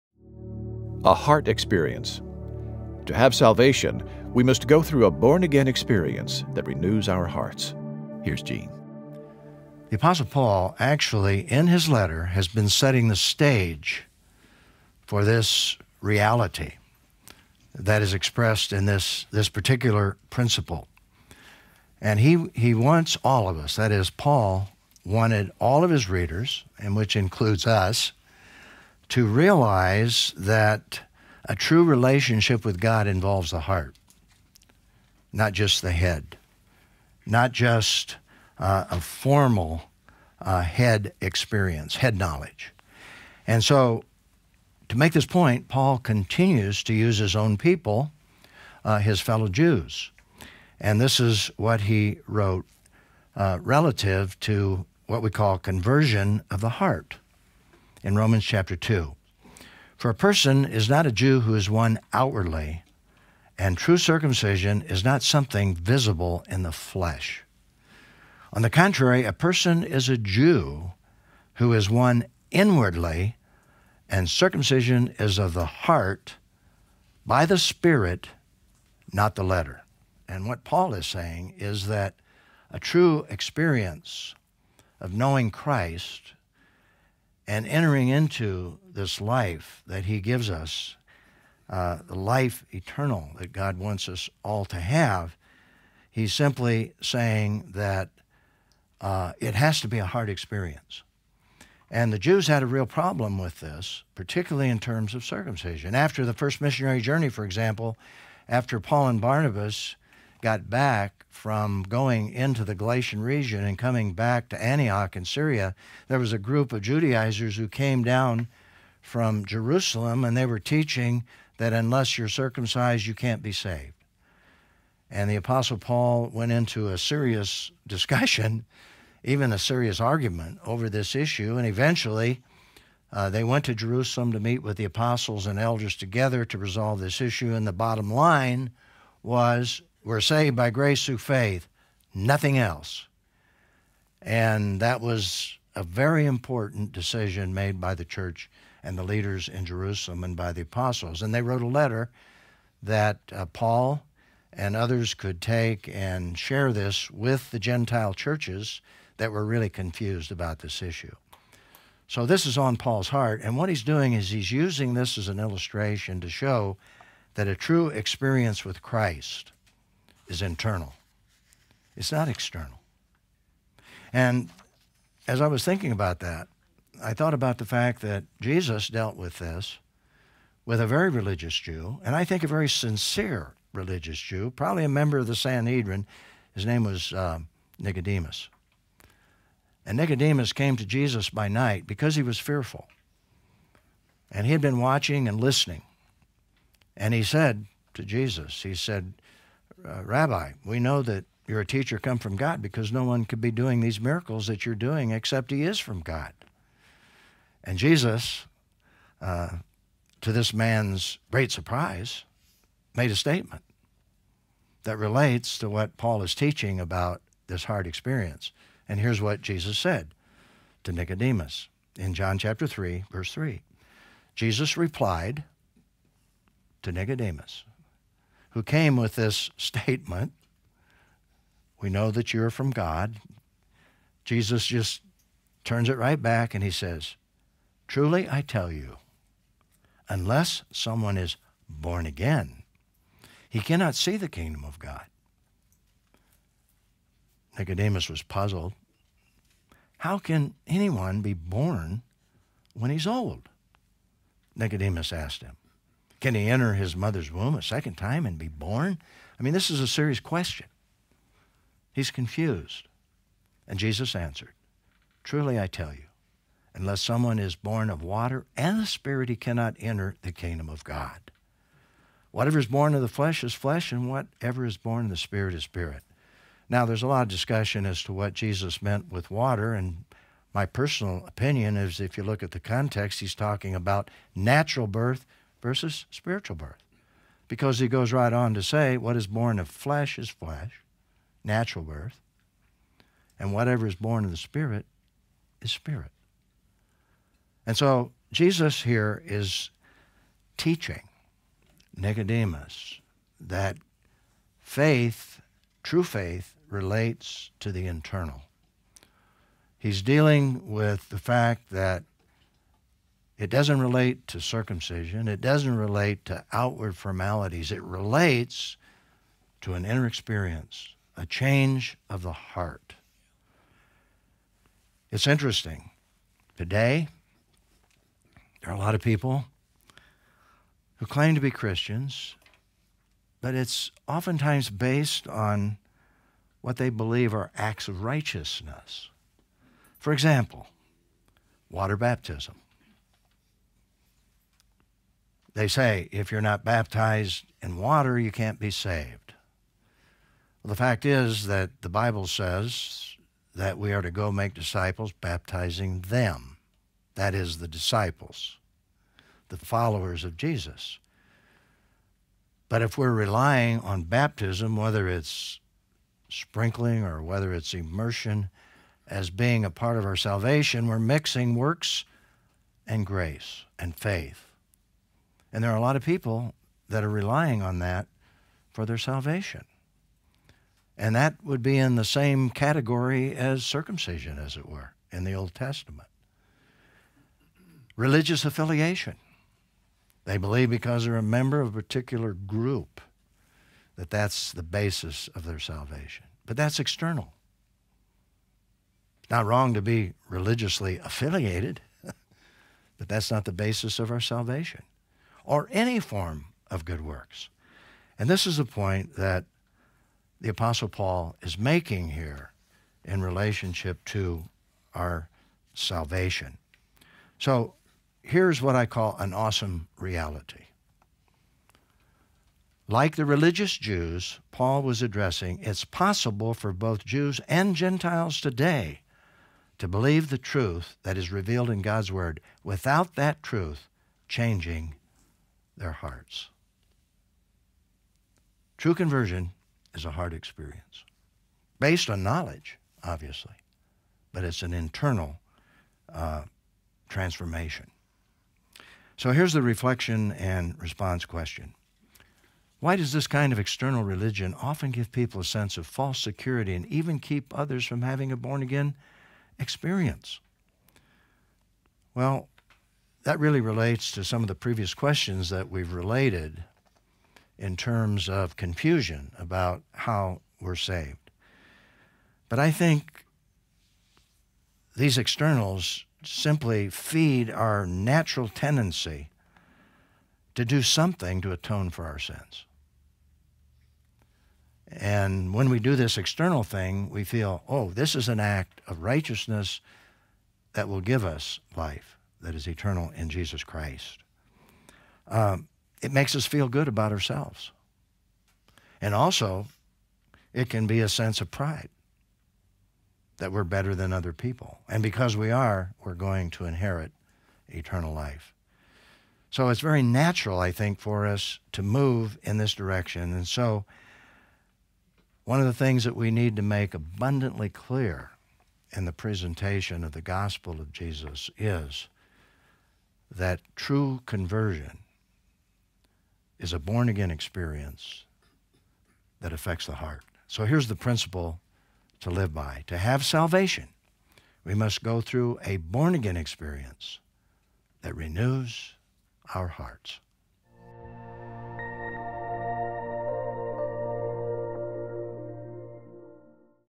Romans Principle 5: A Heart Experience
To have salvation, we must go through a born-again experience that renews our hearts.
Rom 2:25-29, Rom #5, Ro #5, Romans 2, Ro 2